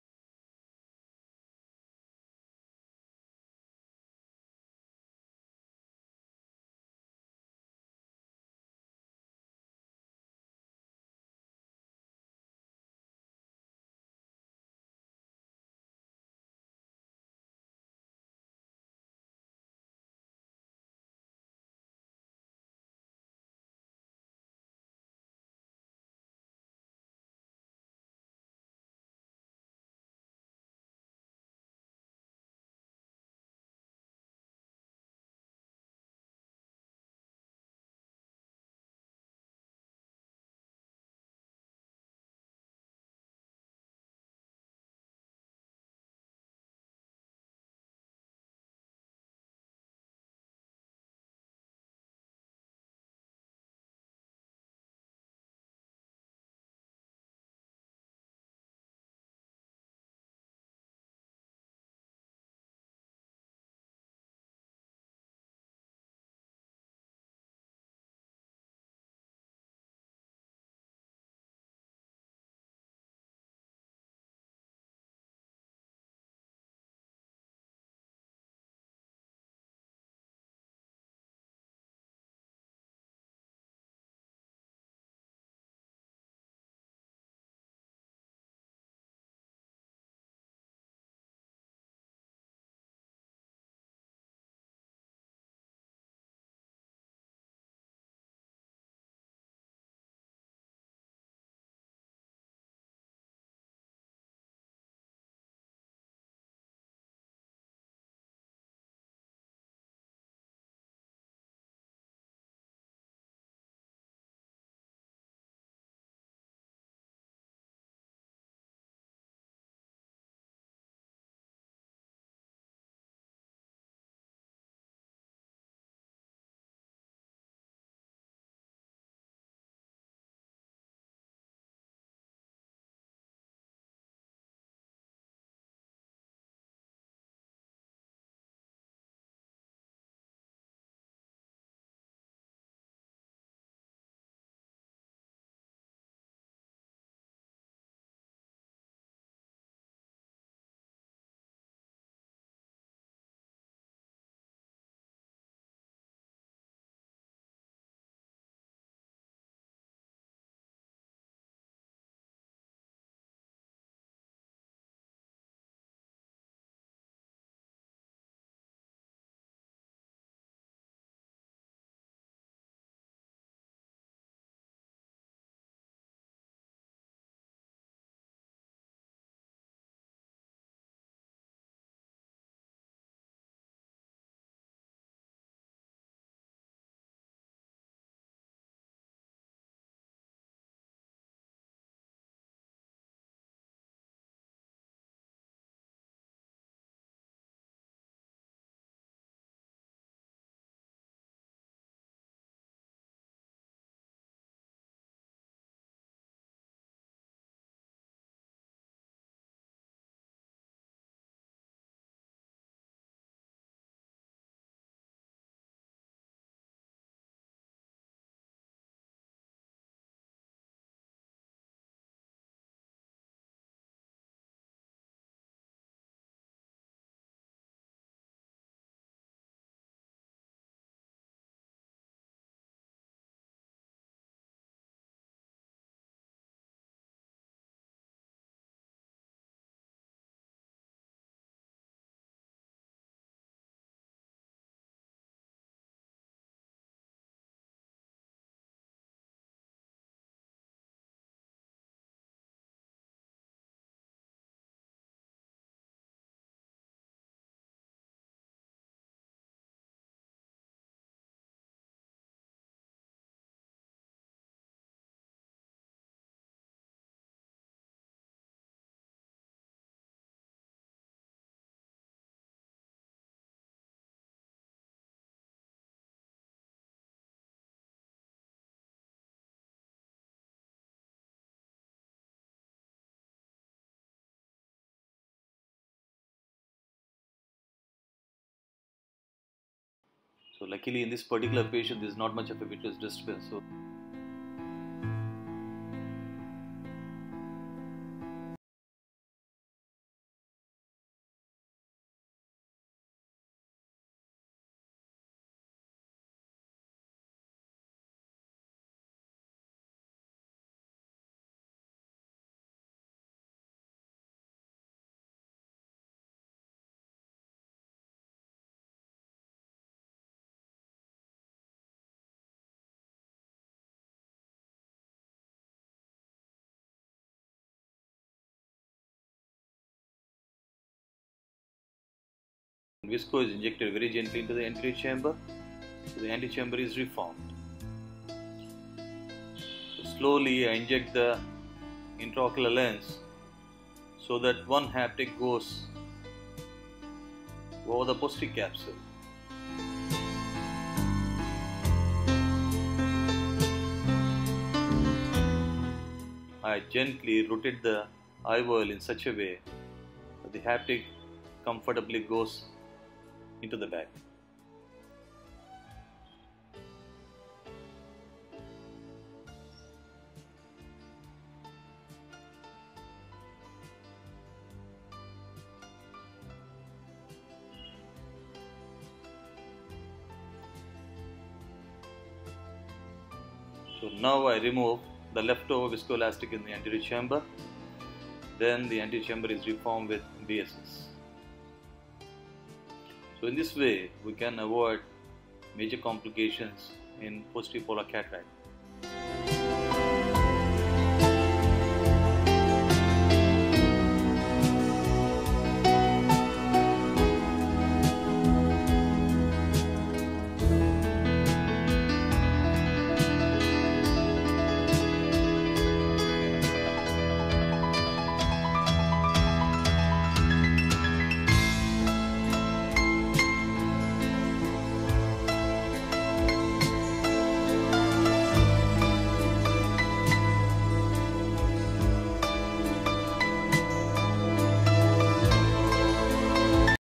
Inside out Hydrodelineation -Phaco in PPC -Dr Ramakrishna Tadanki
This video is about Phaco in Posterior Polar Cataract. An Old video with "voice over"